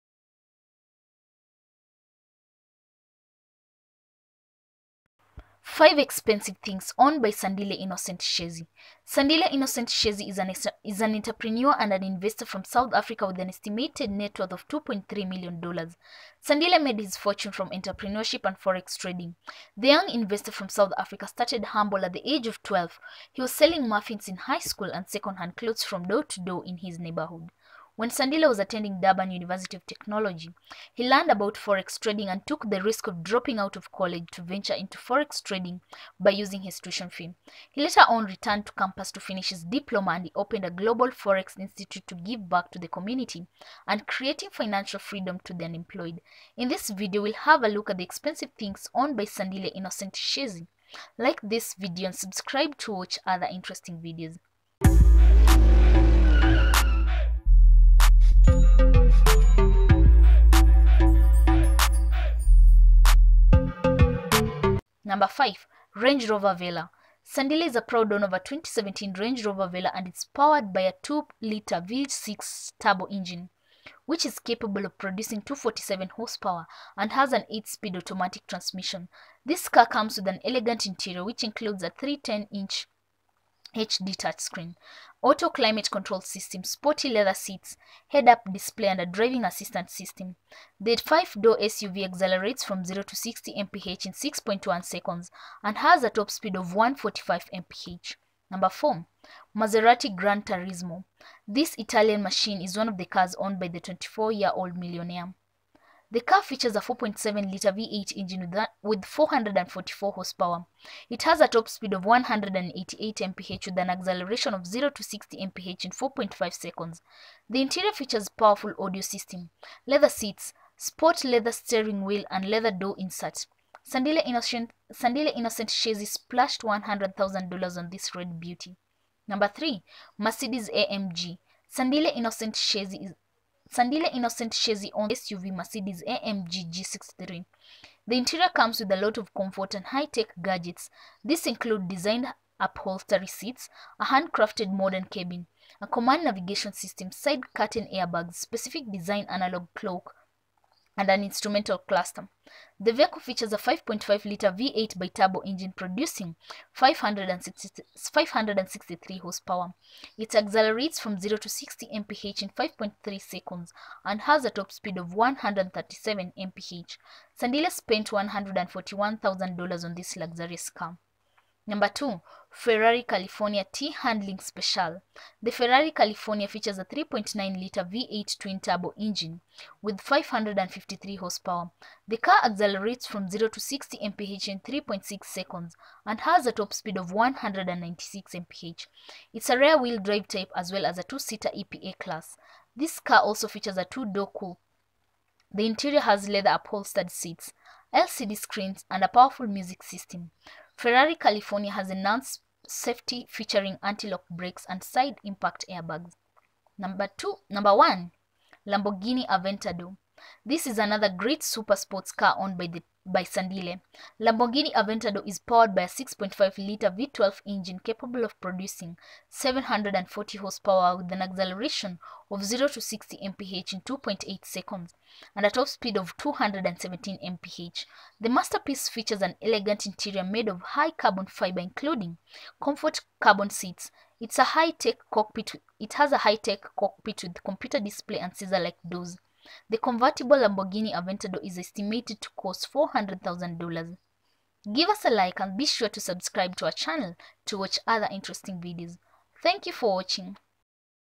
5 Expensive things owned by Sandile Innocent Shezi.
This video brings to you 5 Expensive things owned by Sandile Innocent Shezi. Sandile Innocent Shezi is an entrepreneur and an investor from South Africa with an estimated net worth of $2.3 million. Sandile made his fortune from entrepreneurship and forex trading. The young investor from South Africa started humble at the age of 12; he was selling muffins in high school and second-hand clothes from door to door in his neighbourhood. When Sandile was attending Durban University of Technology, he learned about Forex trading and took the risk of dropping out of college to venture into Forex trading by using his tuition fee. He later on returned to campus to finish his diploma and he opened a Global Forex Institute to give back to the community and creating financial freedom to the unemployed.